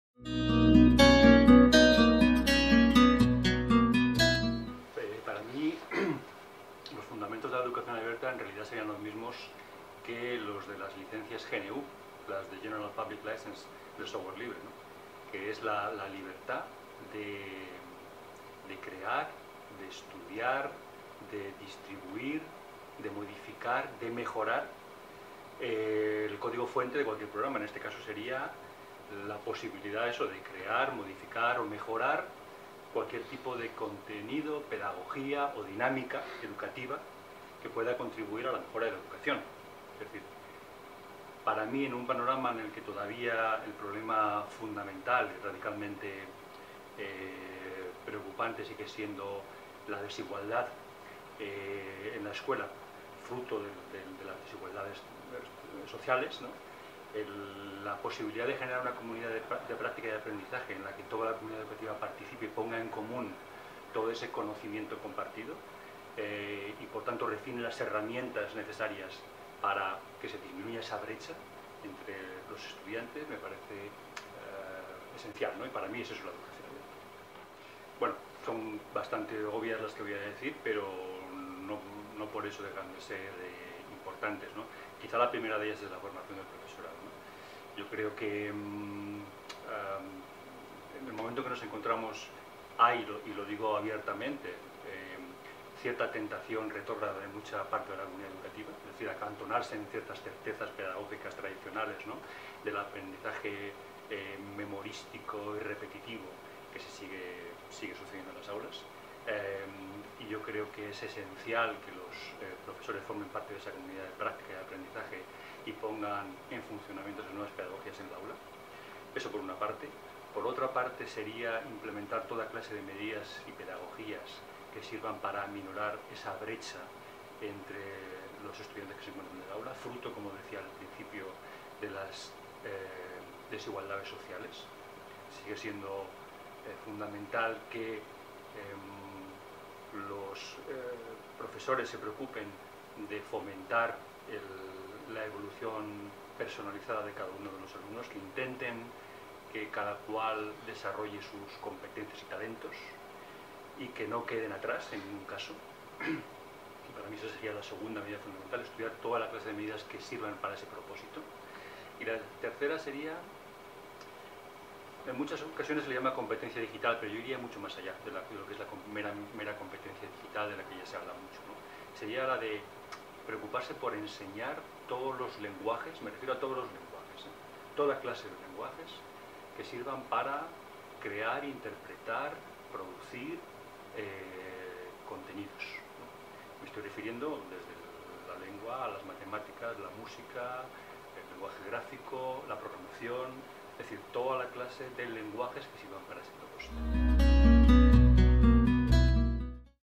Entrevista Joaquin Rodríguez
Entrevista a Joaquin Rodríguez, miembro de la Asociación Educación Abierta